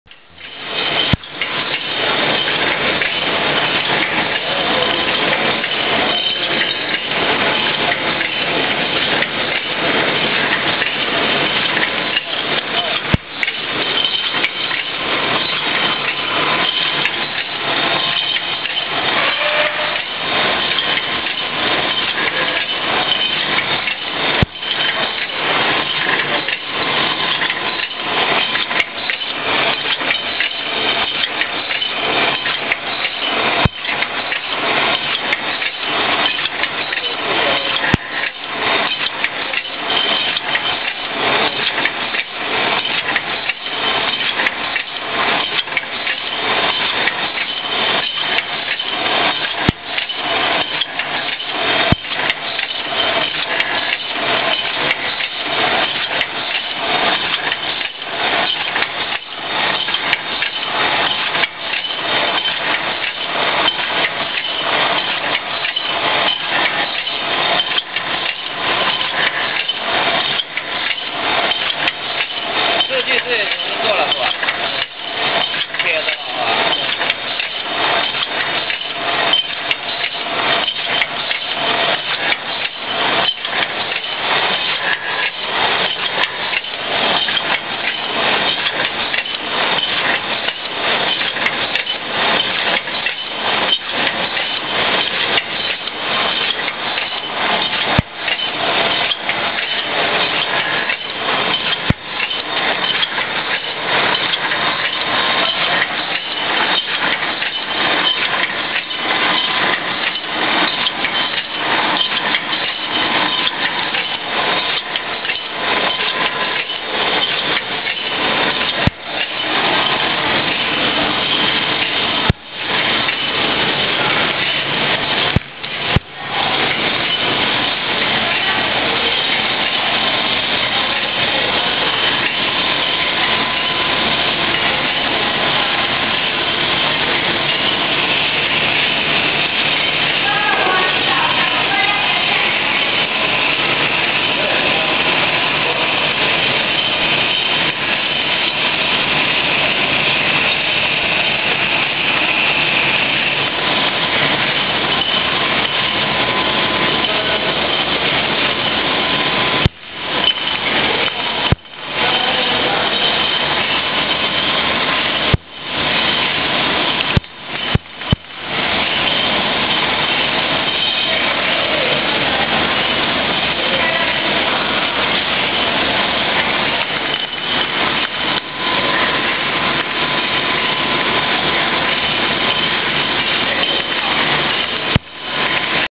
CNC Automatic Multicolor Screen Printing Machine
No any gears
Three set servo Transmission
Two set registration Leuze sensors
Overprint printing accuracy : ±0.1mm

Skype:  printer-manufacture